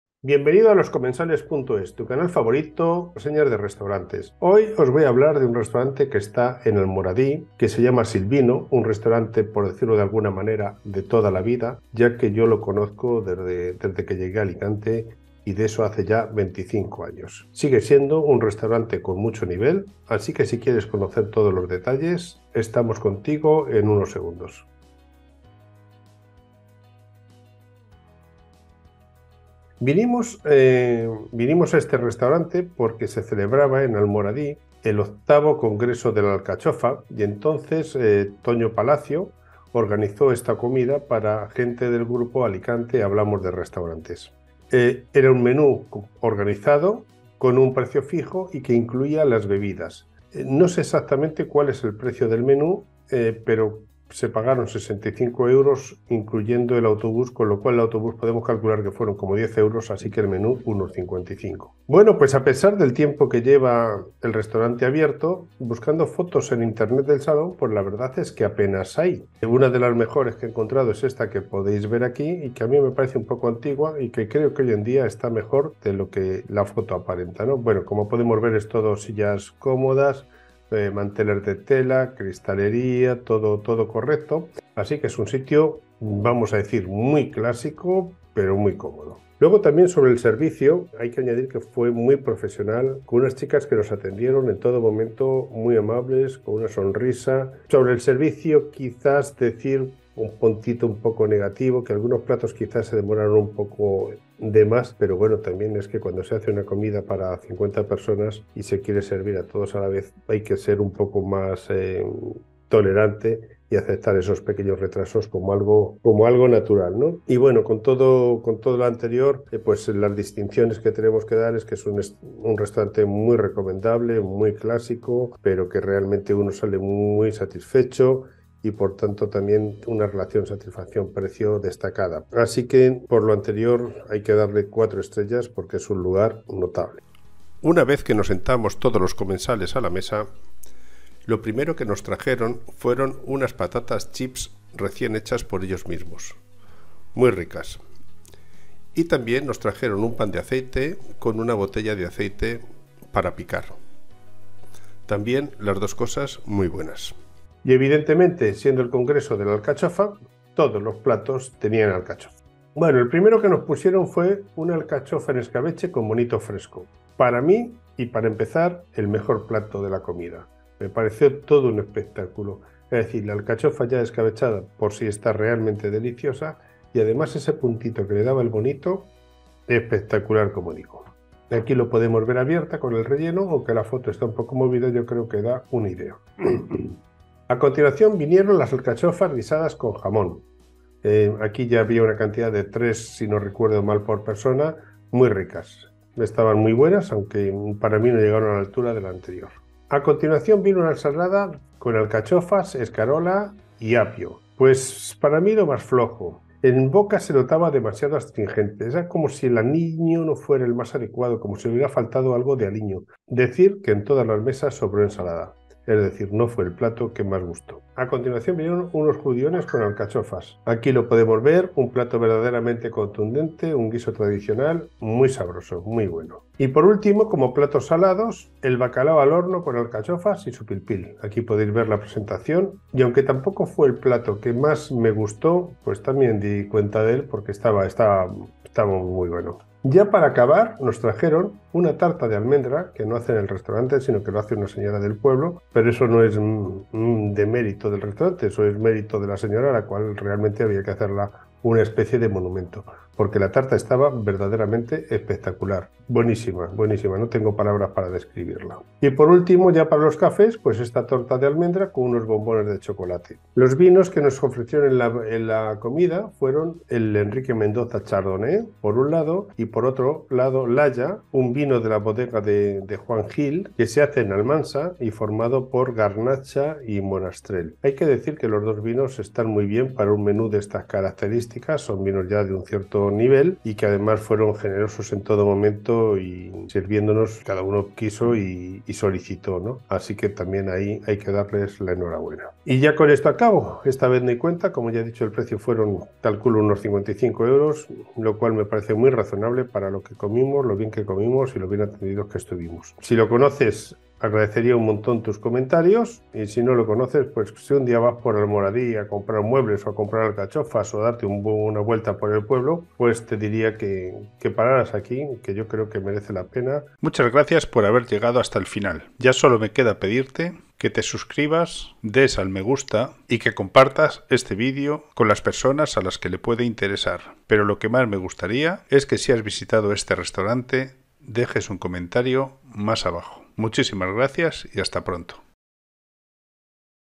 Silvino, Almoradí - Alicante
⭐️⭐️ LÉEME / DESPLIÉGAME ⭐️⭐️

En este vídeo encontrarás la reseña de nuestra visita al Restaurante Silvino en Almoradí (Alicante) en marzo de 2023.

✏️✏️ Contenido de este vídeo ✏️✏️
00:00 Introducción
00:29 El congreso de la alcachofa en Almoradí
01:01 El comedor
01:29 El servicio
01:57 Distinciones y valoración
02:16 Aperitivos: chips, pan y aceite
02:42 Alcachofas en escabeche con bonito fresco
03:07 Alcachofas a la cazuela con jamón
03:19 Ensalada de alcachofas y anchoas en salmuera
03:37 Habichuelas salmantinas con sepia y alcachofas
03:46Bacalao con alcachofas y su pil-pil
03:59 Tarta de almendras
04:18 Café, torta de almendras y bombones
04:23 Los vinos
04:52 Cuenta y conclusiones